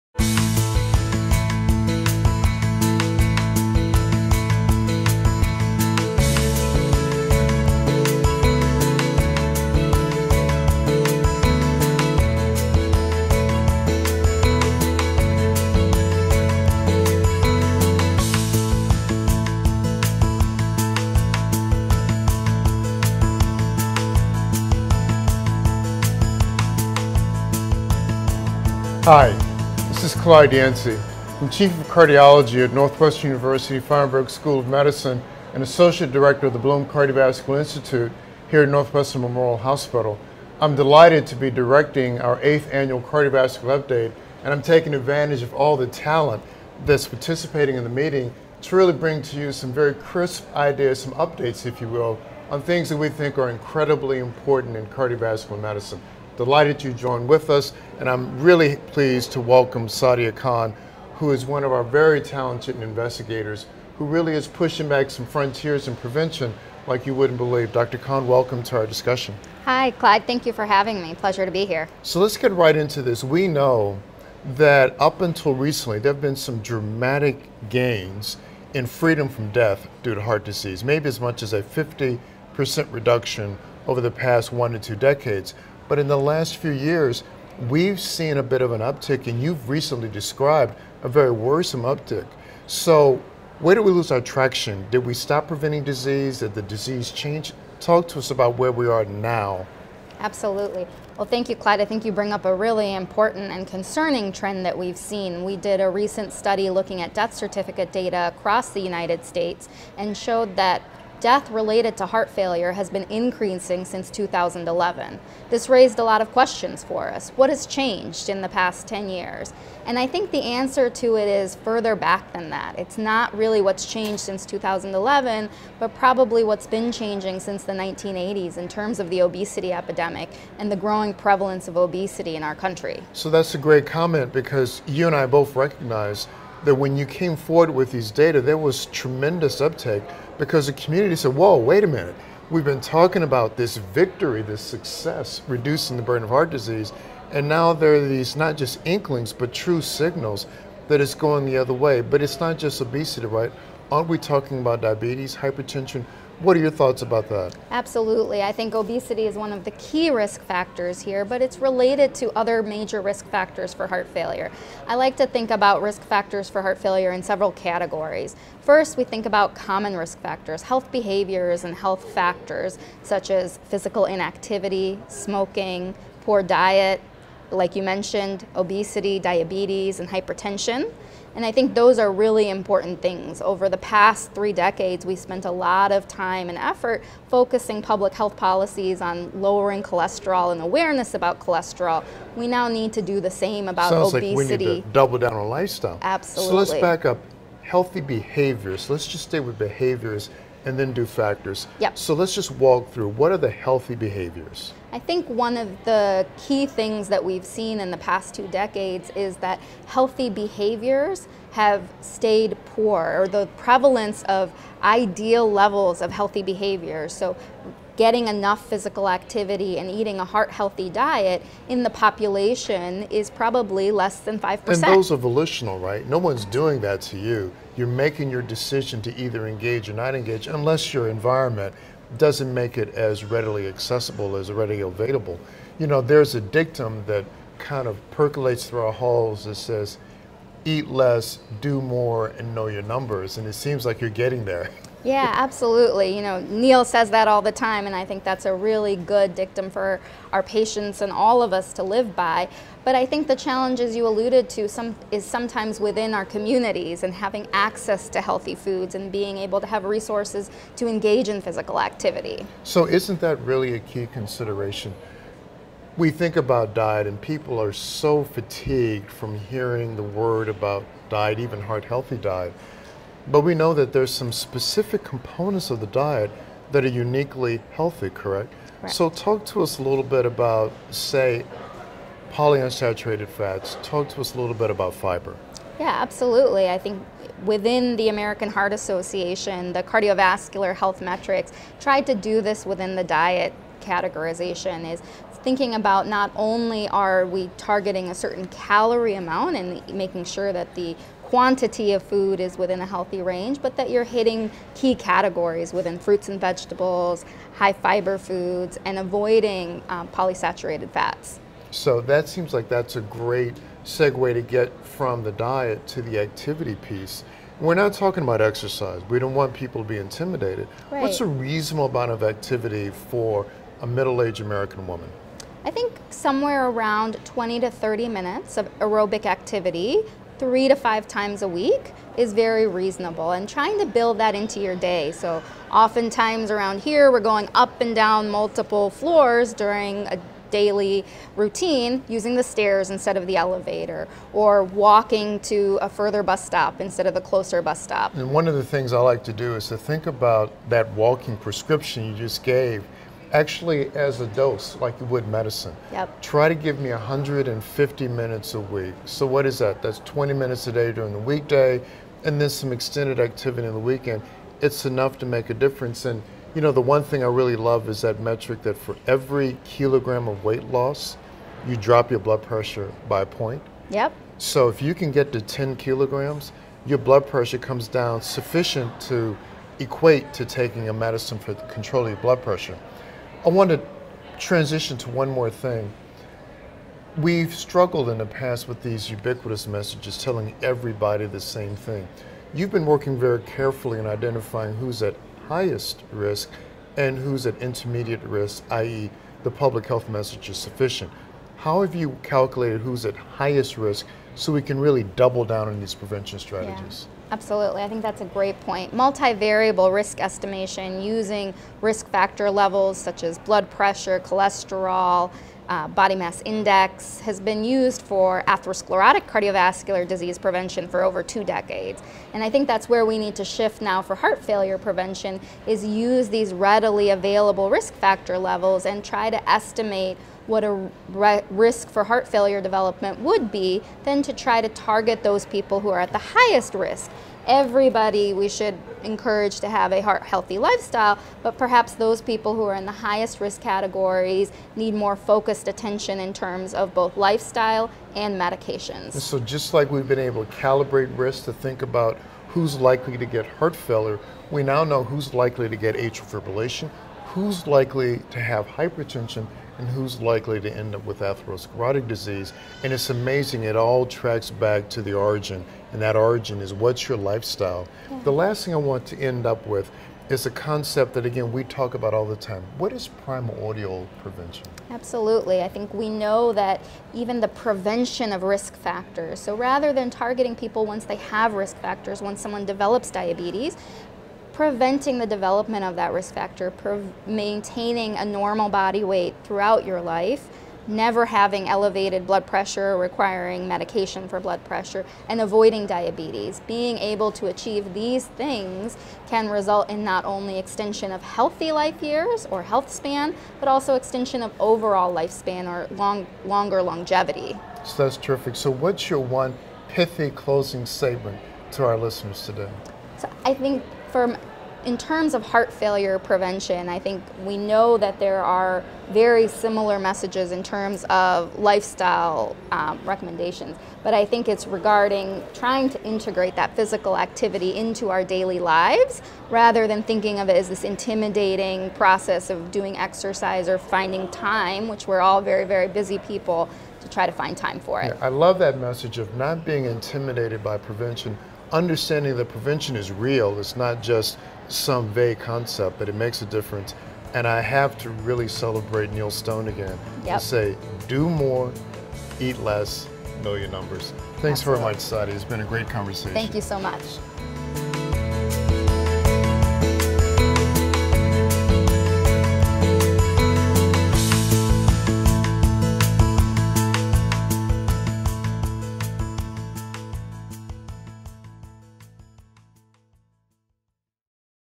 Chicago CVU CME event Heart Failure The New World of Prevention
Clyde W. Yancy, MD, MSc and Sadiya S. Khan, MD, MSc discuss "Heart Failure: The New World of Prevention" at the Northwestern Medicine 8th Annual Chicago Cardiovascular Update CME event.